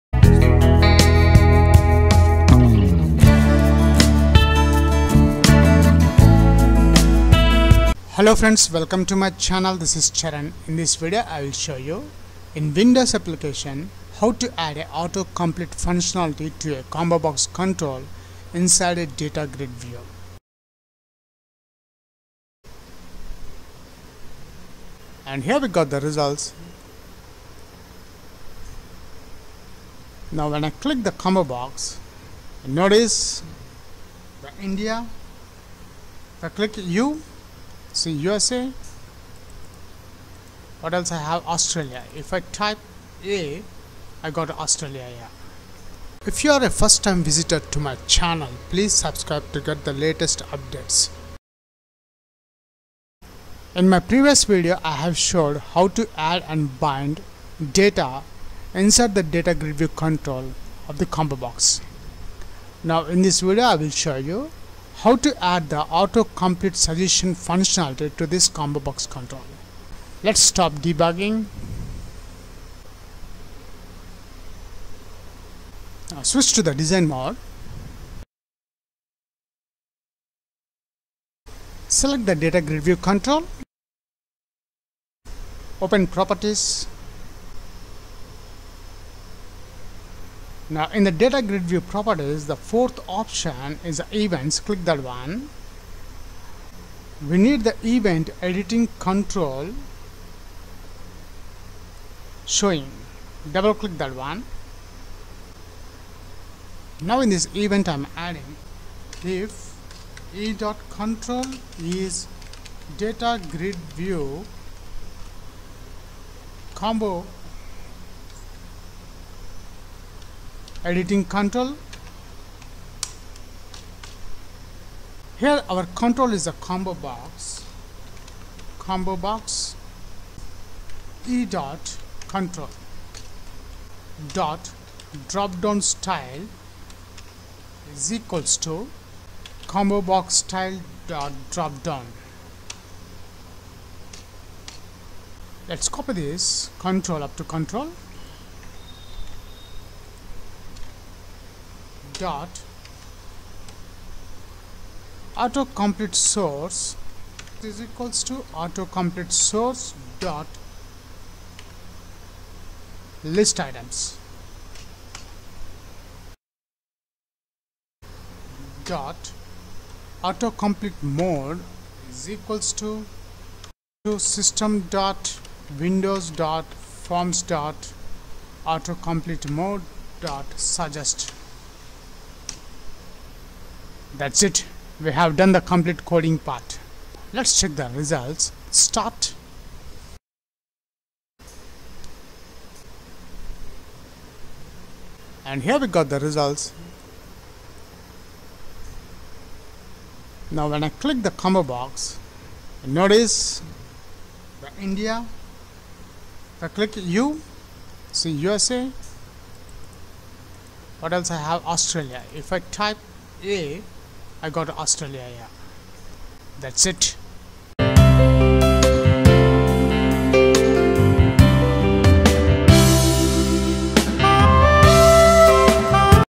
C# Autocomplete Suggestion Combobox Inside Datagridview
In C# windows application, implementing an autocomplete suggestion ComboBox inside a DataGridView involves customizing the DataGridViewComboBoxColumn. By handling the DataGridView's EditingControlShowing event, you can access and modify the ComboBox control within the cell. You'll bind it to a data source and set up autocomplete functionality by handling its TextChanged event, dynamically filtering suggestions as the user types. This enhances user experience by providing real-time suggestions in a DataGridView column with ComboBoxes, facilitating efficient data entry and selection.

combobox autocomplete functionality inside a datagridview in winforms using c# code